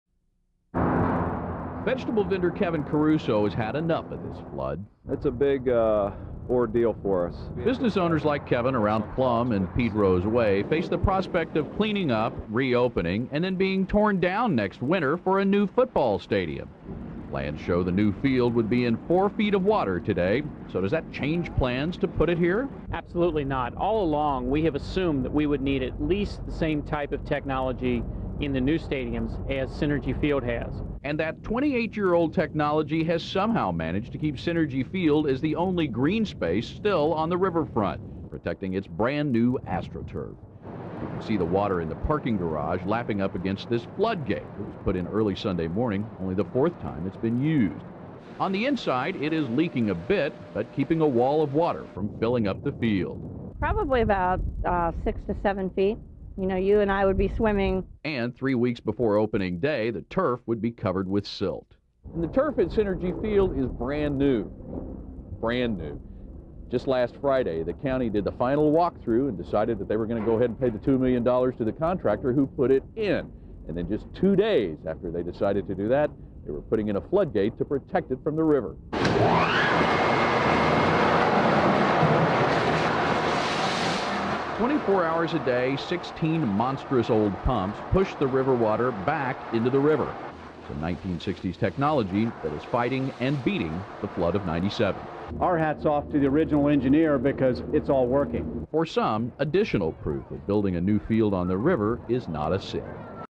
Flood of 1997: How did Riverfront Stadium field stay dry?
Three weeks before Opening Day, the flooded Ohio River had just about filled the parking garage and surrounded Riverfront Stadium. A brand-new $2 million AstroTurf field had been installed, and only some 1960 era water pumps